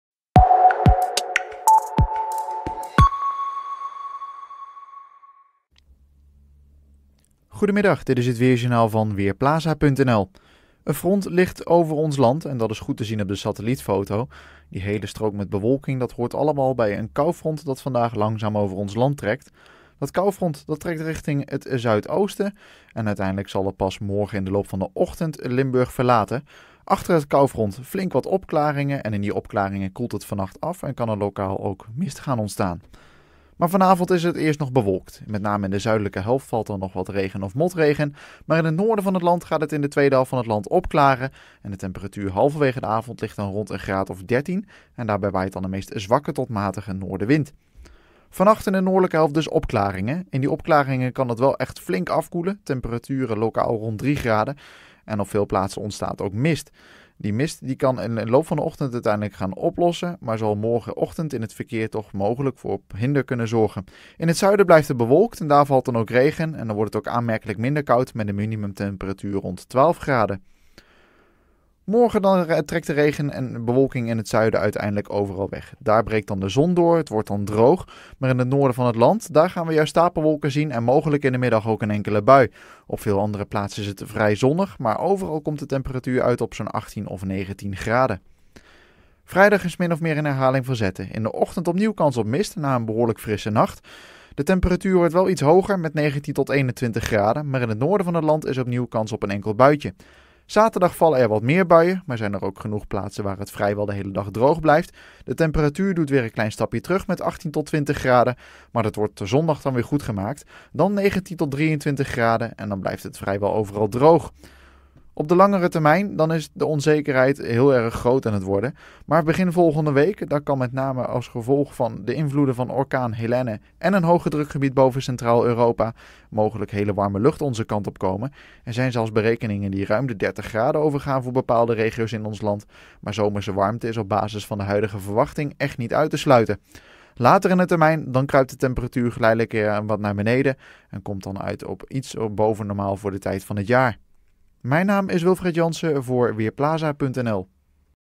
Weerbericht woensdagmiddag: vannacht regionaal mist
Vanmiddag is het bewolkt. Nu en dan valt er regen of motregen. In Limburg schijnt eerst nog de zon, later wordt het ook daar bewolkt. In de neerslag ligt het kwik rond 15 graden, in Limburg kan het ruim 20 graden worden. Vanavond en vannacht trekt de regen verder zuidwaarts. In het noorden klaart het op en er kans mist ontstaan. De temperatuur daalt naar 3 tot 12 graden. Morgen trekken regen en bewolking via het zuiden het land uit. Daarna is het op veel plaatsen wisselend bewolkt en grotendeels droog. De temperatuur ligt rond 18 graden en de noordenwind is zwak.